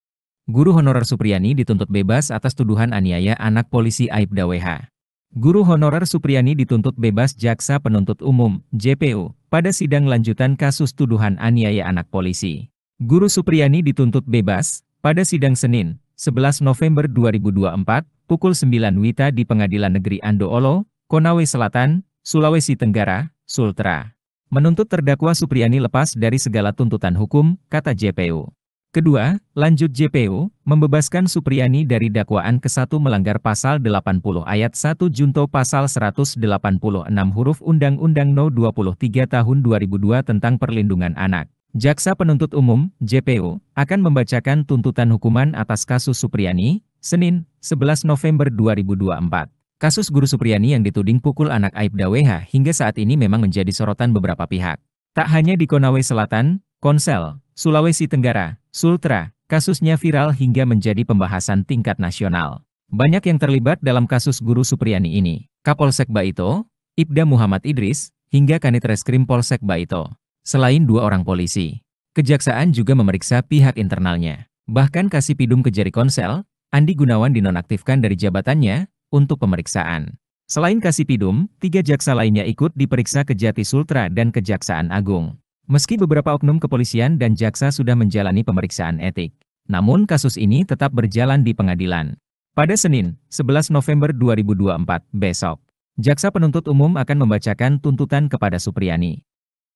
Supriyani Dituntut Bebas, Usai Dituduh Aniaya Anak Polisi Aipda WH
Guru honorer Supriyani dituntut bebas Jaksa Penuntut Umum (JPU) pada sidang lanjutan kasus tuduhan aniaya anak polisi.

Editor Video: Randa